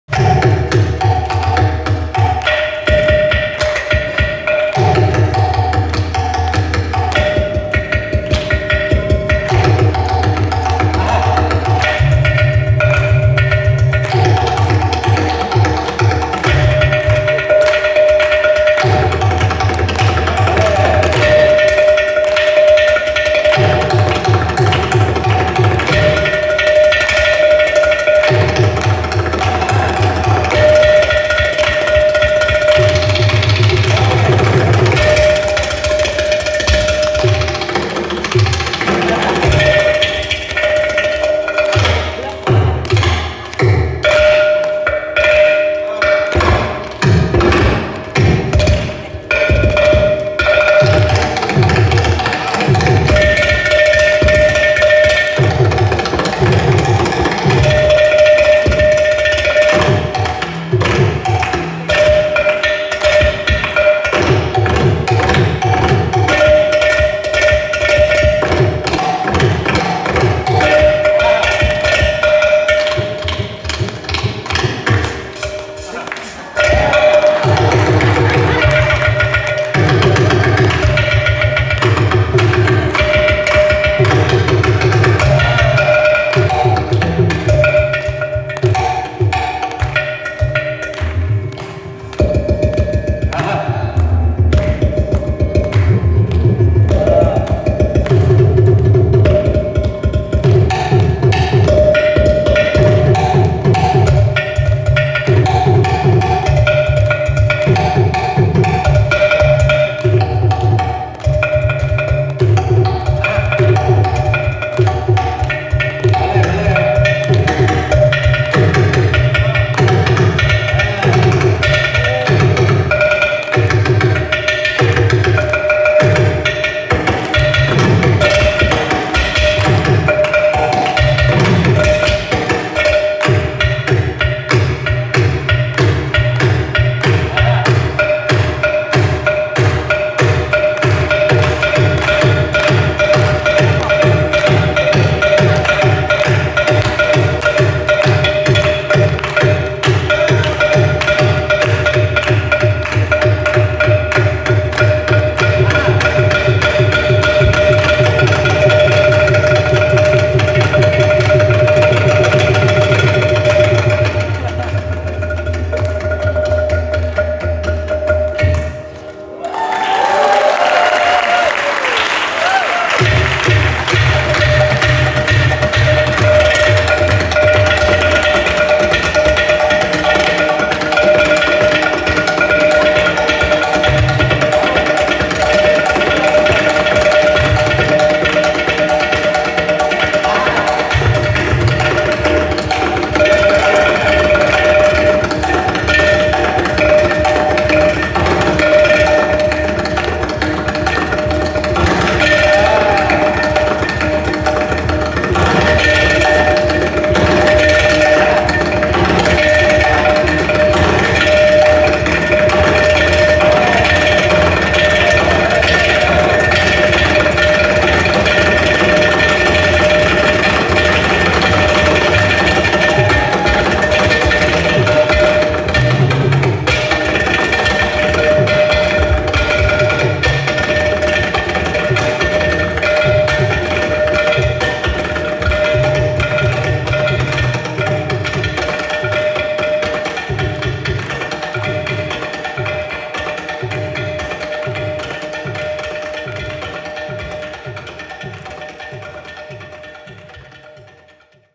Anantha R Krishnan | Giridhar Udupa | Mridangam | Ghatam | Tani Avarthana
It was an absolute joy performing this concert with my most favourite musician and my dearest friend Vidwan Anantha R Krishnan.